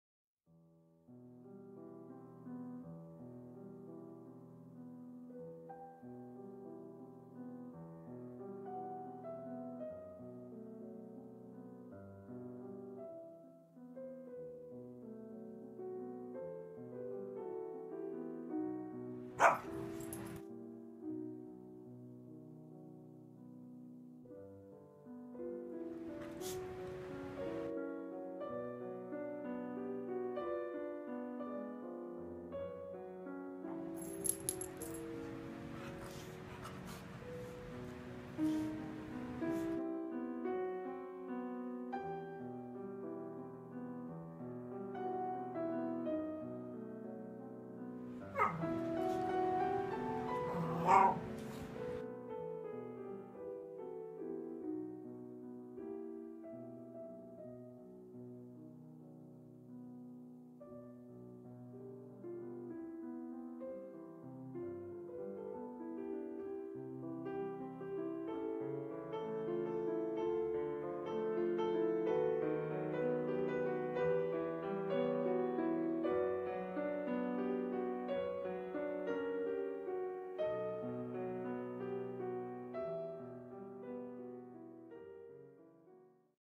Dublin the Wheaten: The World's Cutest Dog
This is a little video I threw together of my dog Dublin rolling around and making funny noises. She does this very often but I've only recently been able to catch her on film.